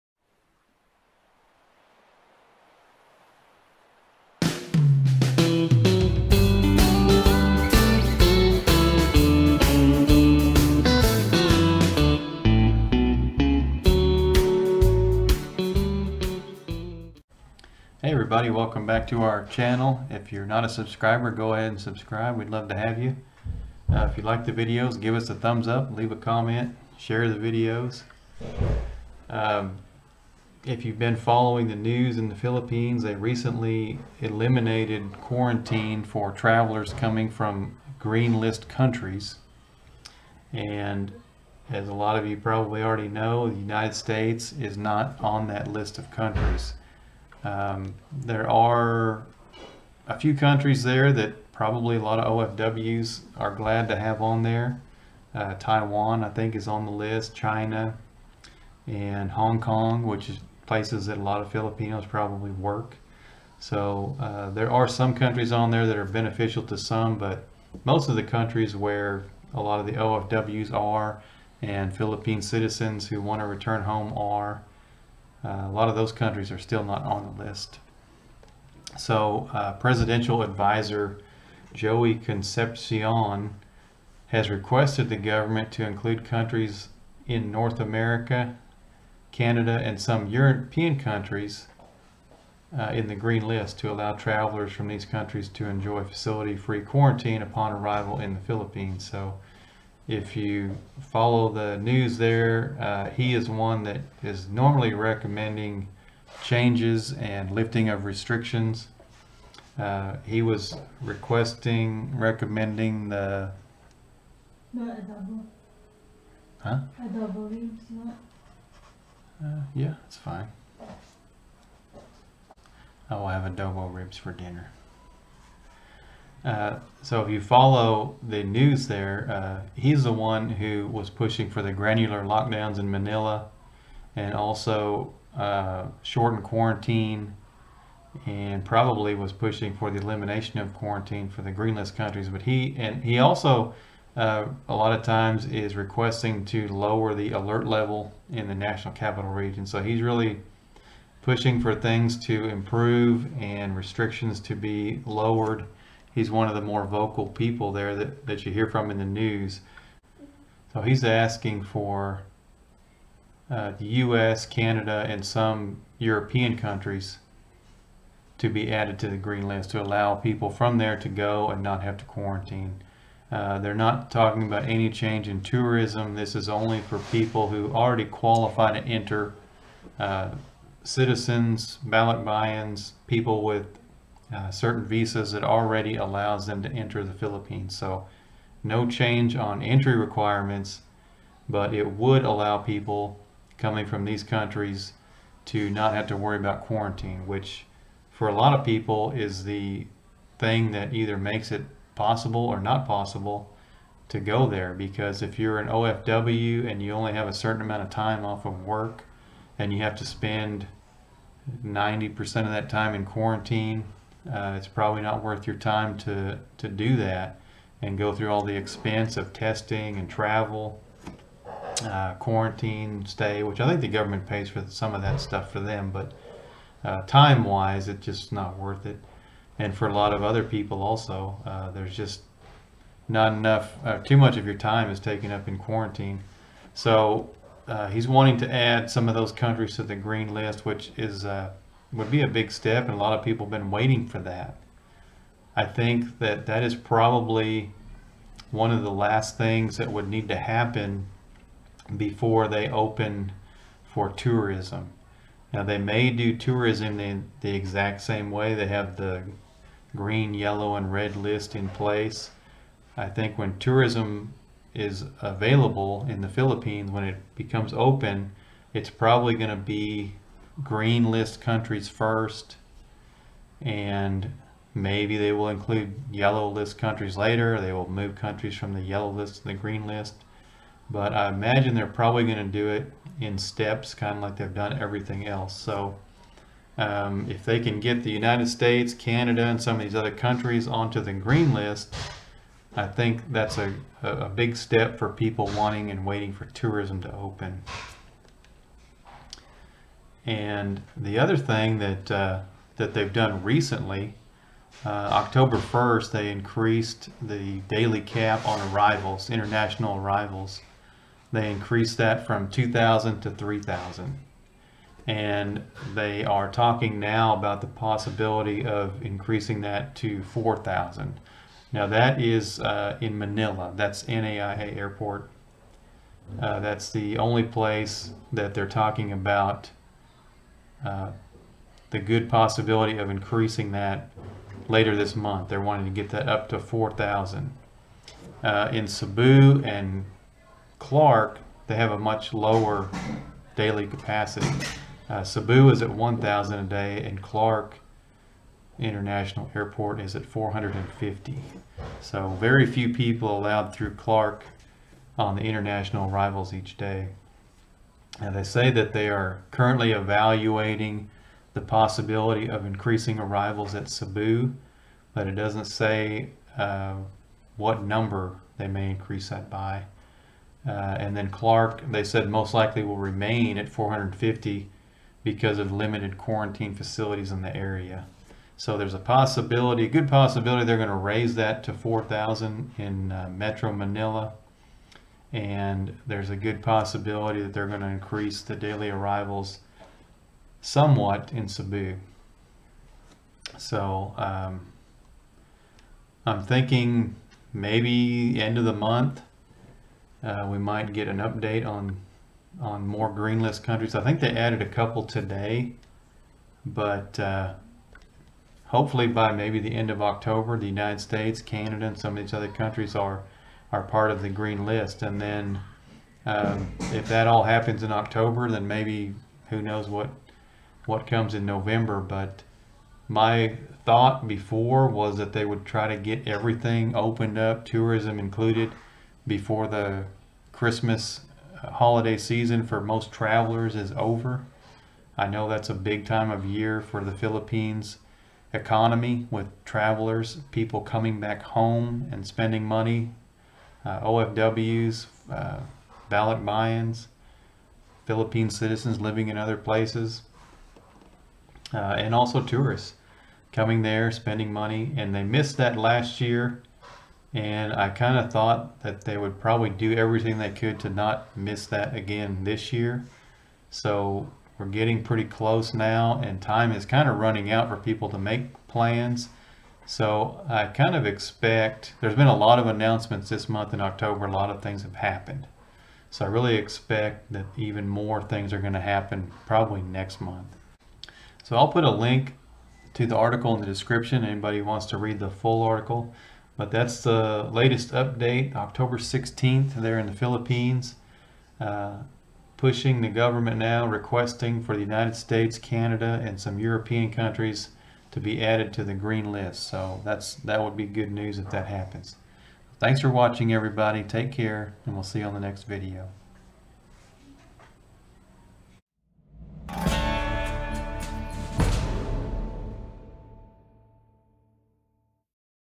Philippines Travel Update: US, Canada, and Europe ADDED to GREEN LIST soon?
WE APPRECIATE IT VERY MUCH!!